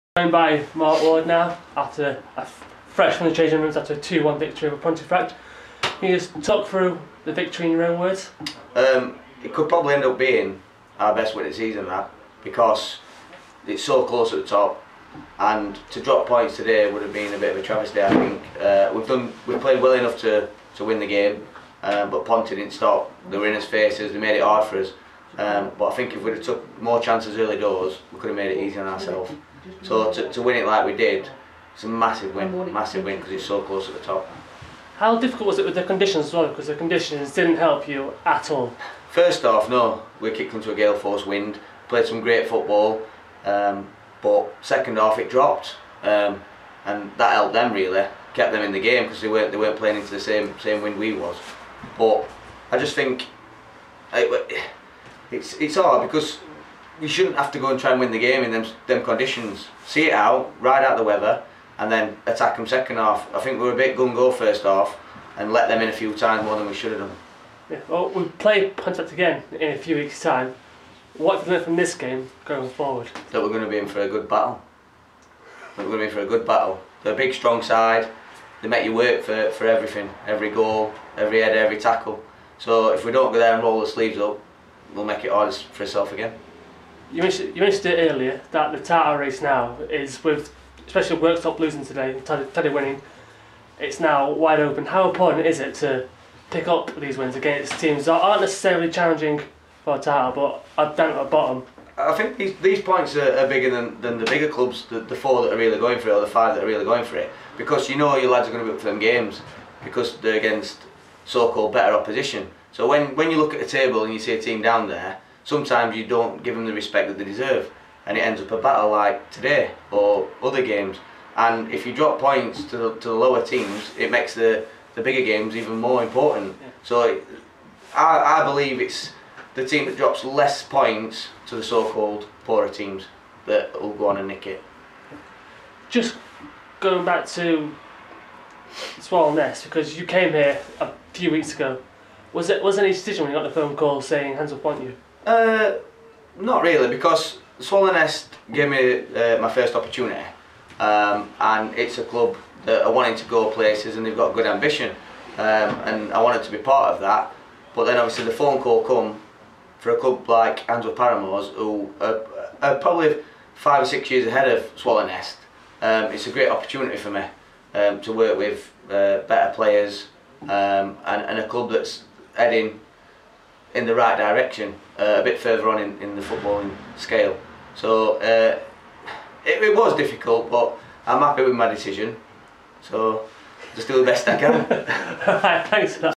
Interview: Mark Ward (Ponte H)
Mark Ward gives his views on the win against Pontefract, the title race and moving to Handsworth Parramore